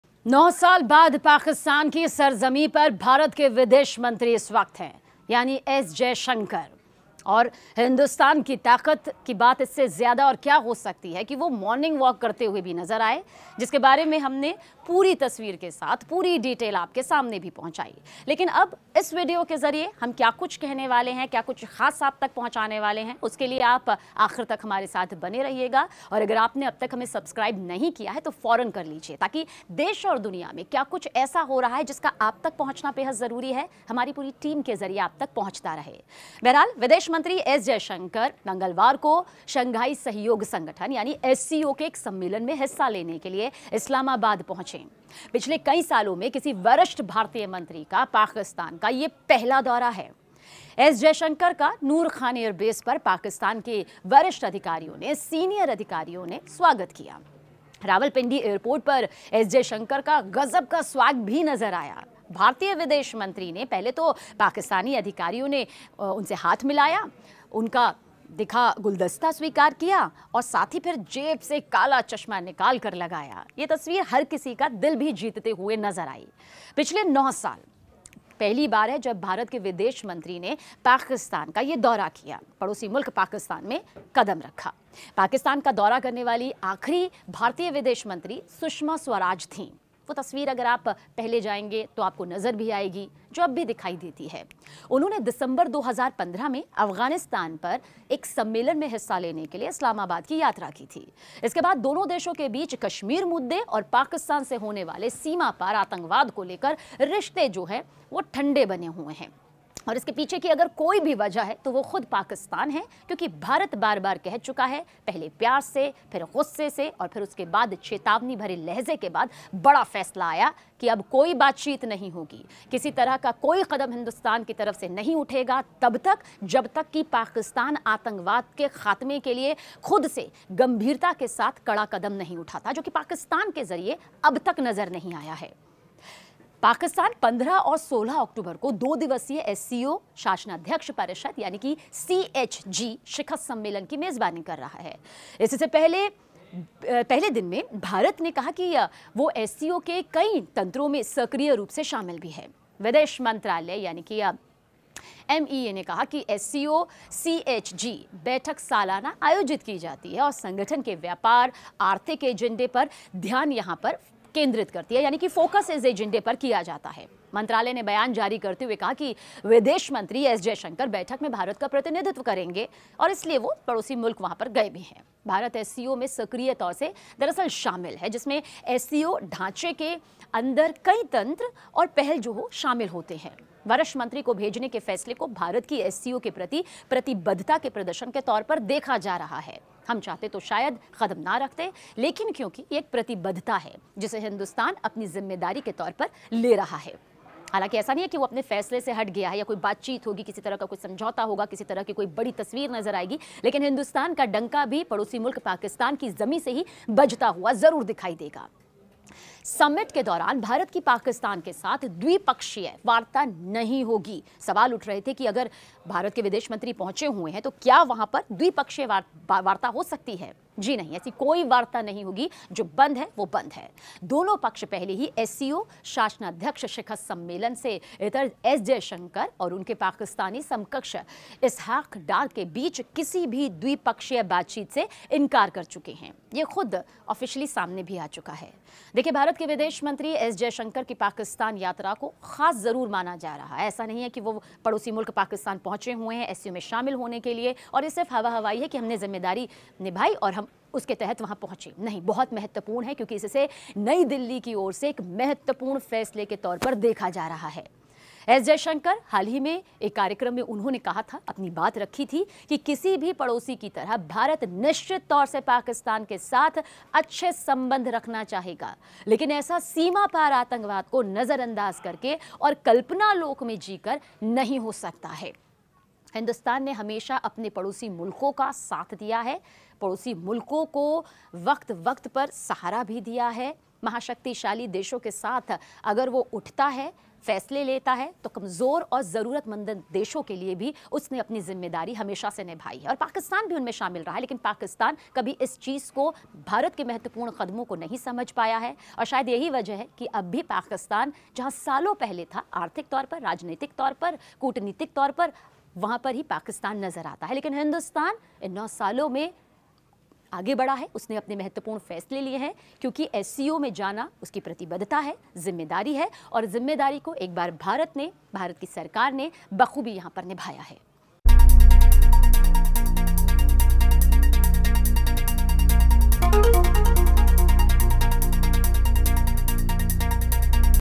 Jaishankar in Pakistan Update :पाकिस्तान की सरजंमी में दिखा जयशंकर का स्वैग| SCO Summit 2024
एससीओ समिट के लिए पाकिस्तान पहुंचे भारतीय विदेश मंत्री एस जयशंकर का नूर खान एयरबेस पर सीनियर अधिकारियों ने स्वागत किया. वहां एयरपोर्ट पर जयशंकर का गजब का स्वैग दिखा. आप भी देखें...